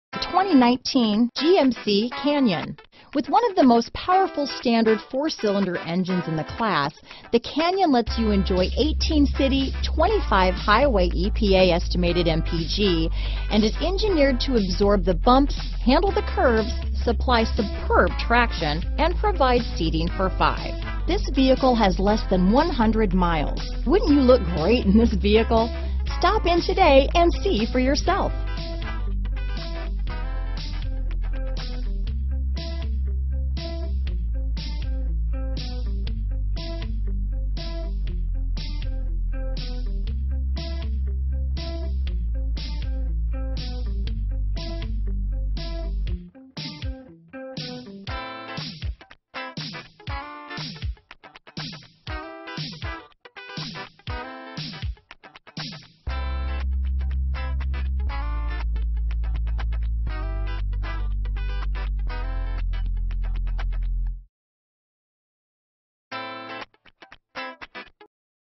2019 GMC Canyon Ocala, The Villages, Brooksville, Chiefland, Homosassa FL N19433
Onyx Black  2019 GMC Canyon available in Homosassa, Florida at Eagle Buick GMC. Servicing the Ocala, The Villages, Brooksville, Chiefland, FL area.
2019 GMC Canyon 2WD Denali - Stock#: N19433 - VIN#: 1GTG5EEN5K1298527

Onyx Black 2019 GMC Canyon Denali RWD 8-Speed Automatic V6  Recent Arrival!